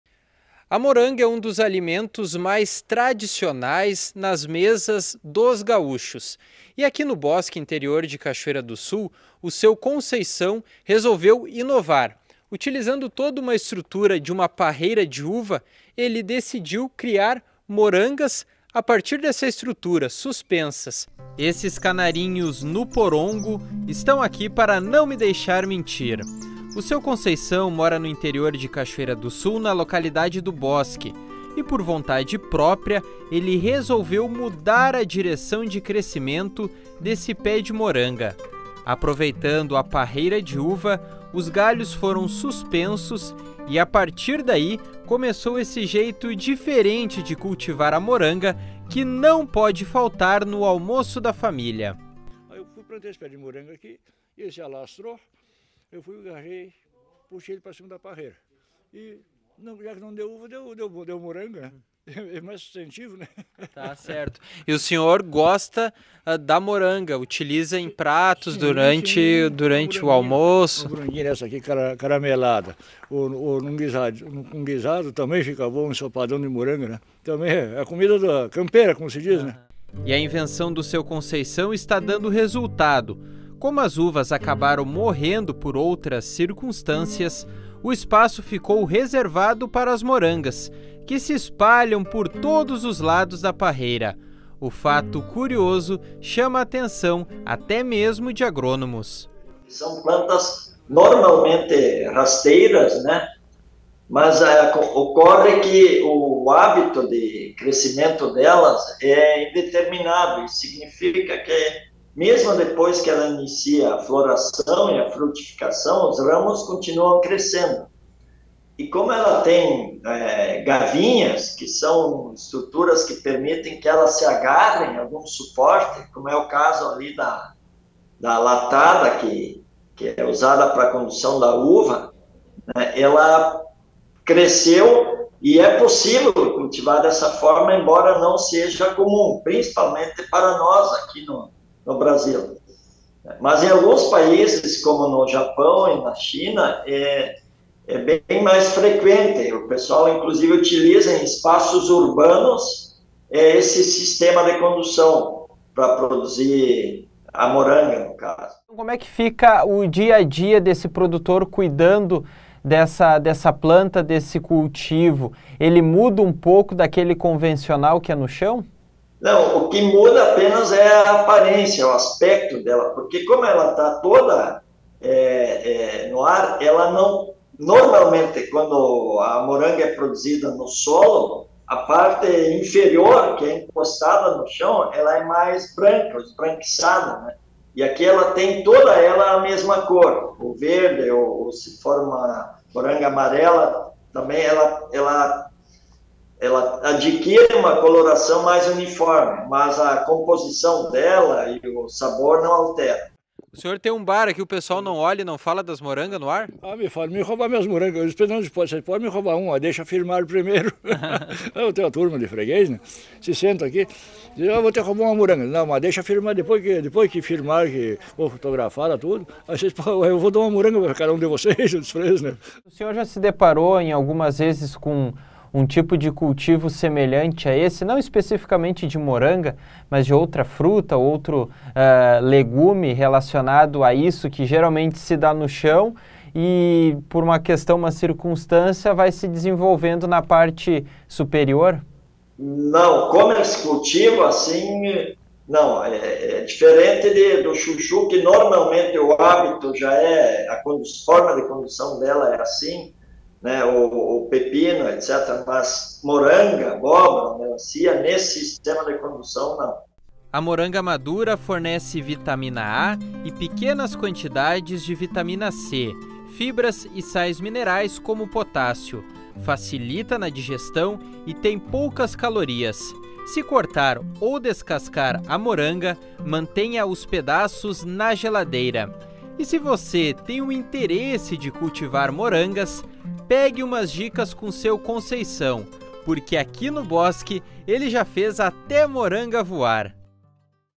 Parreira de morangas vira atração no interior de Cachoeira do Sul
A inovação foi do seu Conceição que não deixa ninguém colher os frutos.